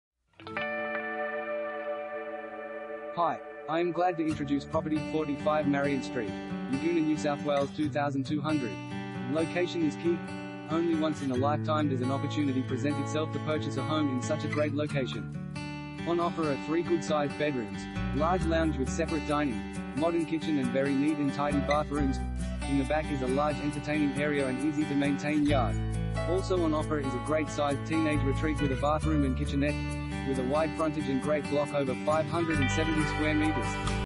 45 Marion Street, Yagoona NSW 2200 | AuPropertyReport.Com
45 Marion Street, Yagoona NSW 2200
Property Type: house
Bedrooms: 3
Bathrooms: 3
Car Space: 2
Location Is Key